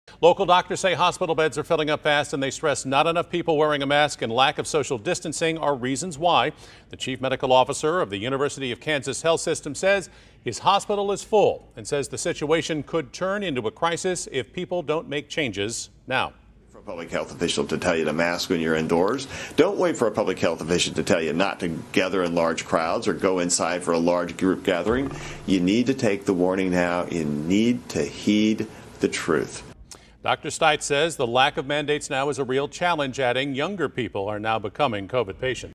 University of Kansas Health System says its hospital beds are full
Kansas City doctors say hospital beds are filling up fast saying not enough people are wearing masks and social distancing.